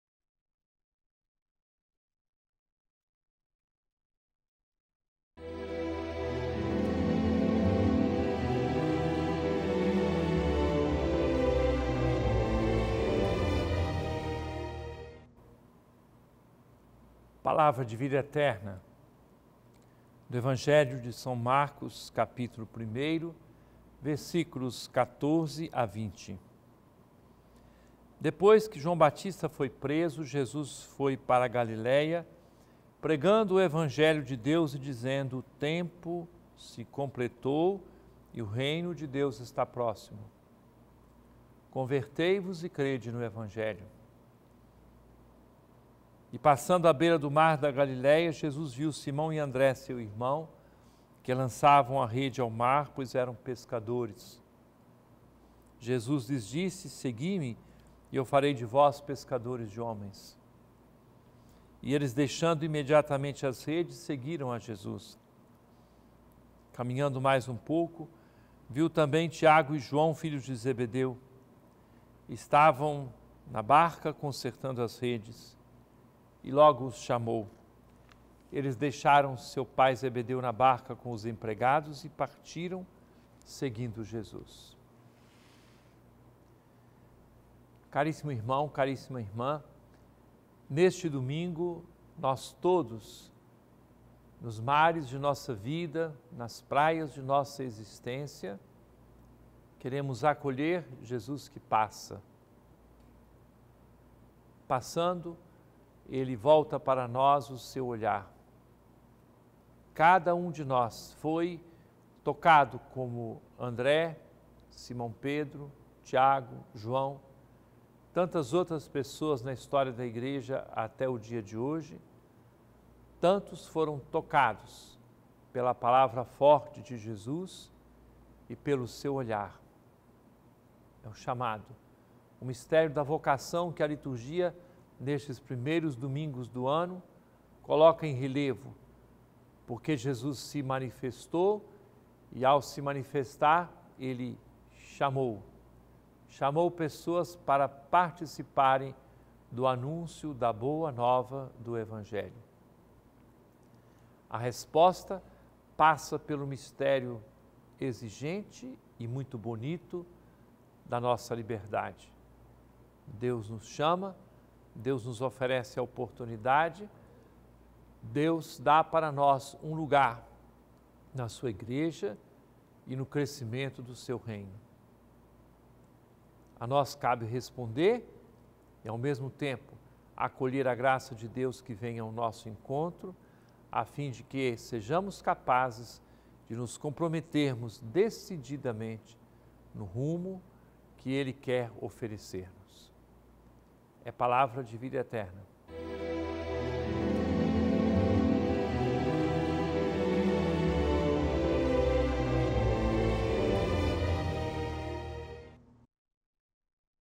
Palavra de Vida Eterna | São Marcos 1,14-20 21/01/2018
14Depois que João Batista foi preso, Jesus foi para a Galileia, pregando o Evangelho de Deus e dizendo: 15“O tempo já se completou e o Reino de Deus está próximo. Convertei-vos e crede no Evangelho!”

16E, passando à beira do mar da Galileia, Jesus viu Simão e André, seu irmão, que lançavam a rede ao mar, pois eram pescadores.

17Jesus lhes disse: “Segui-me e eu farei de vós pescadores de homens”.

18E eles, deixando imediatamente as redes, seguiram a Jesus.

19Caminhando mais um pouco, viu também Tiago e João, filhos de Zebedeu. Estavam na barca, consertando as redes; 20e logo os chamou. Eles deixaram seu pai Zebedeu na barca com os empregados, e partiram, seguindo Jesus.